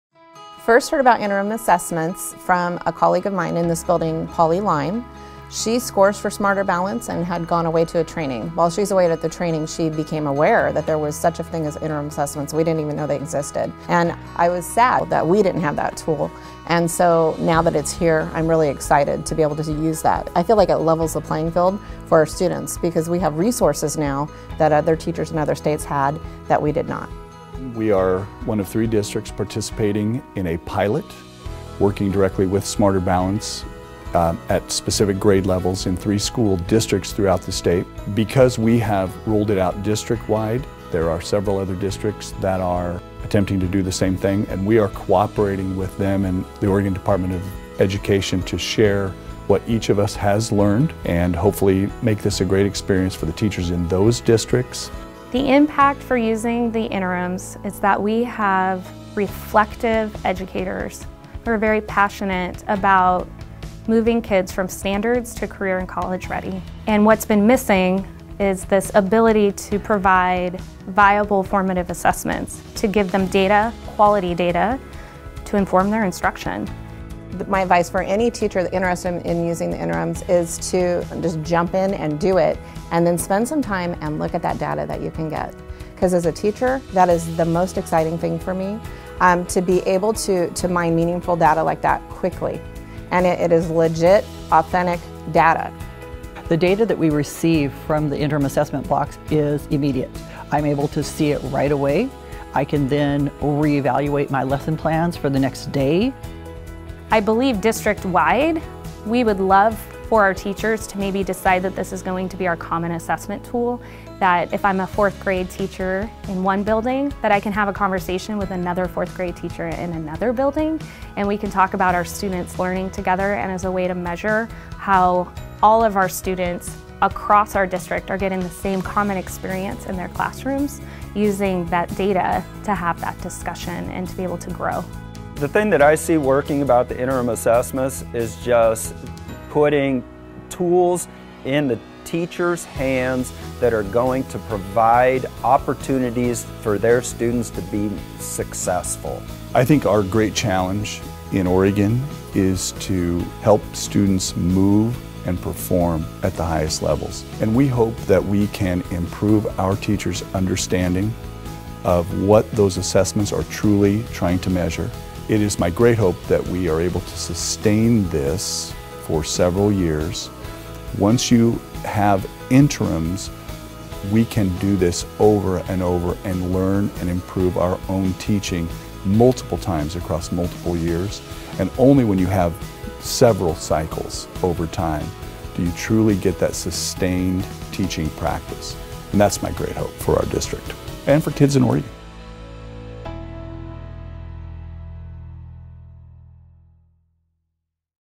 Interims Level the Playing Field for Students in North Santiam School District (OR)
Three schools in North Santiam School District (OR) are piloting the use of Smarter Balanced interim assessments and are finding that the power of data is transforming instruction. Hear from teachers and leadership about their experience taking students from standards to college and career-ready.